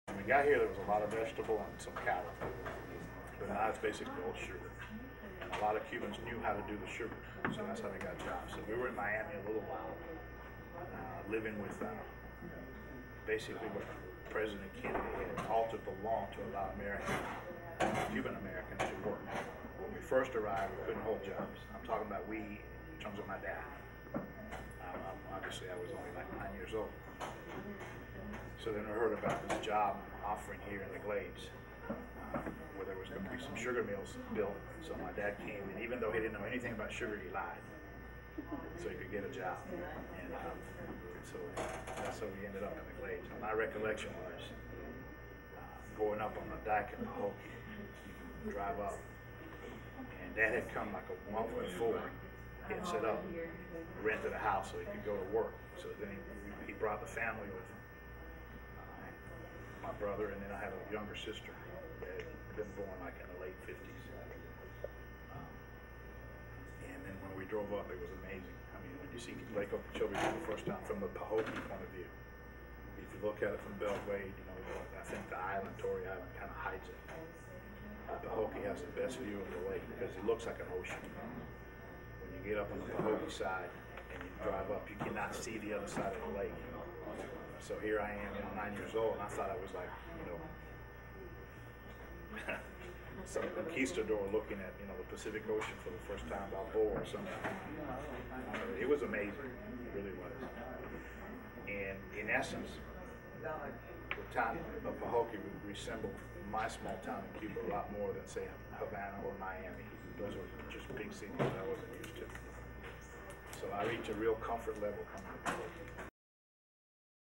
Hispanic immigration to U S and Lake Okeechobee in Florida
Cuban Armando Perez recalls first encounter with U.S. as a young immigrant in the 1960s.  Excerpt from cross-cultural archival Race and Change Oral Histories,  a project directed by Dr. Kitty Oliver .